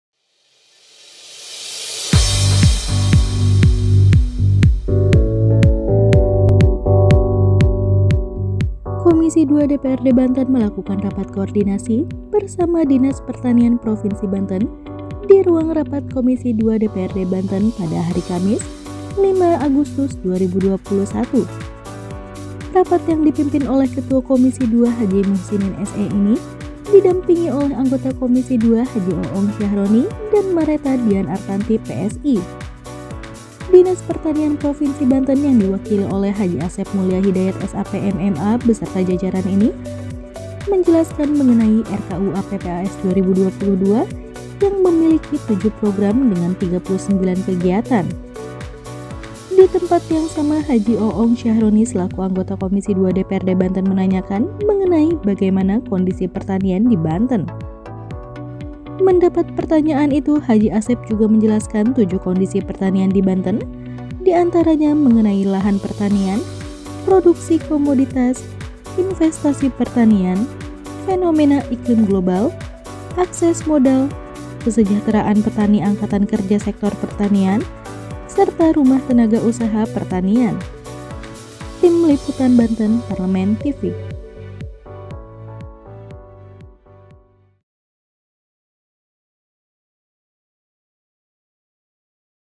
Rapat Koordinasi (RaKor) Komisi II Bersama Dinas Pertanian
Komisi II Mengadakan Rapat Bersama Dengan Dinas Pertanian di Ruang Rapat Komisi II Pada Tanggal 05 Agustus 2021